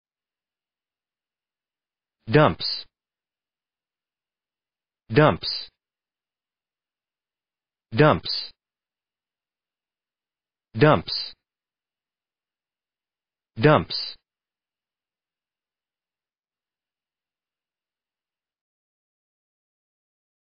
How to pronounce dumps
dumps American English pronunciation. How to pronounce dumps correctly. How to say dumps in proper American English.